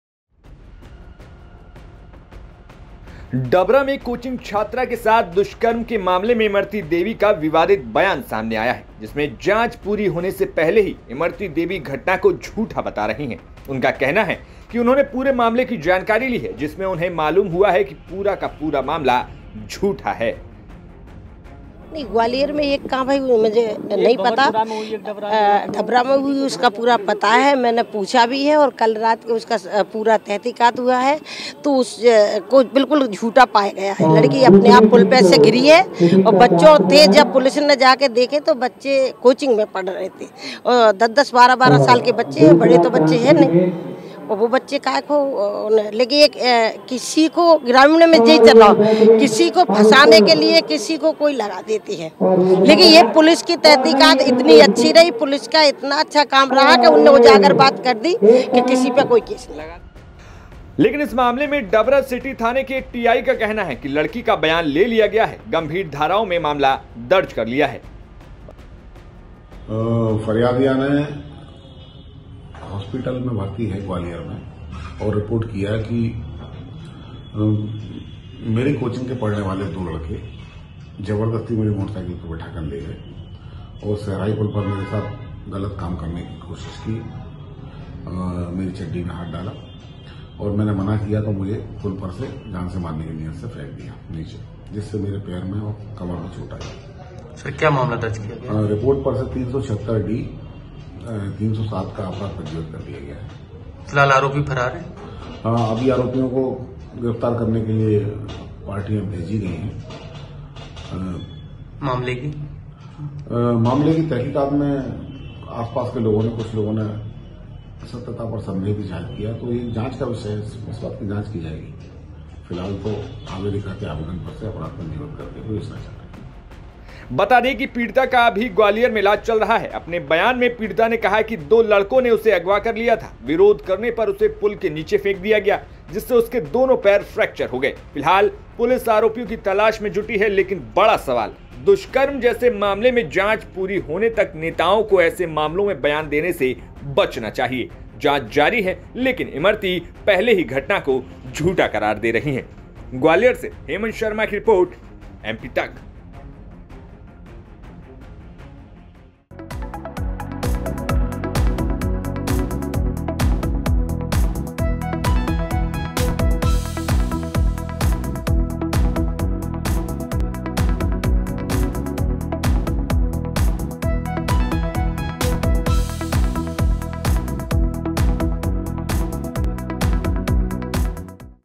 Scindia समर्थक पूर्व मंत्री Imarti Devi ये बयान देकर फिर आ गई विवादों में ? MP Tak
Dabra में कोचिंग छात्रा के साथ दुष्कर्म के मामले में Imarti Devi का विवादित बयान सामने आया है. जिसमे जांच पूरी होने से पहले ही इमरती देवी घटना को झूठा बता रही है.उनका कहना है कि उन्होंने पूरे मामले की जानकारी ली.जिसमें उन्हें मालूम हुआ कि पूरा मामला झूठा है.देखें रिपोर्ट.